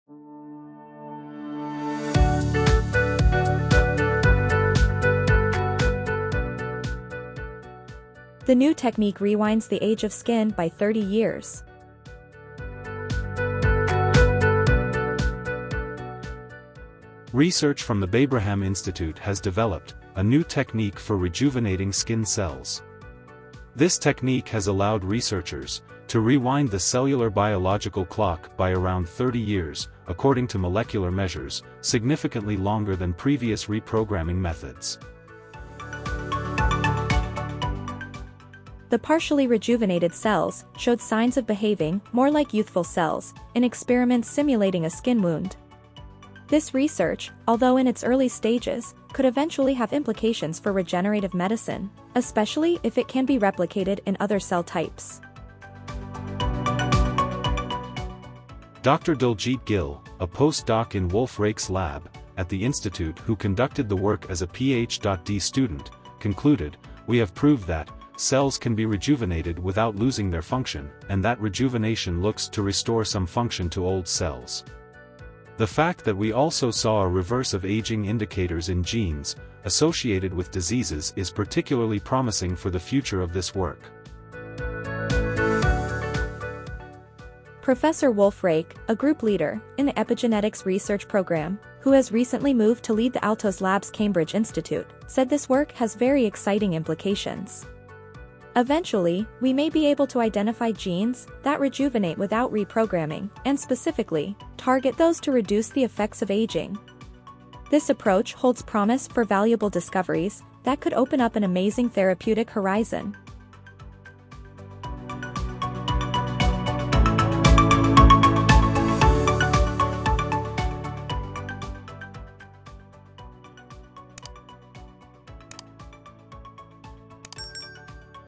The new technique rewinds the age of skin by 30 years
Research from the Babraham Institute has developed a new technique for rejuvenating skin cells.
This technique has allowed researchers to rewind the cellular biological clock by around 30 years according to molecular measures, significantly longer than previous reprogramming methods.
The partially rejuvenated cells showed signs of behaving more like youthful cells in experiments simulating a skin wound.